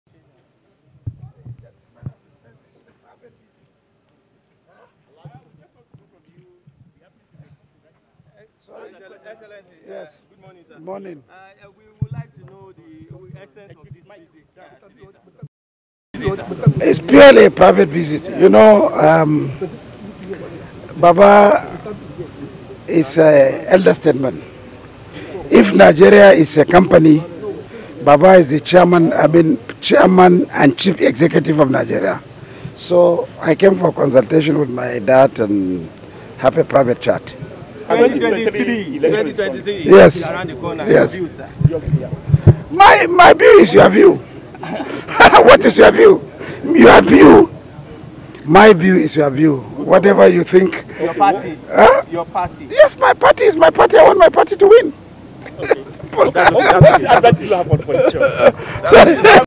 Obasanjo Is Chairman, Chief Executive Officer Of Nigeria - Ali Modu-Sheriff
Former governor of Borno state, Senator Ali Modu-Sheriff  has described former President Olusegun Obasanjo as the Chief Executive Officer and chairman of the federal Republic of Nigeria.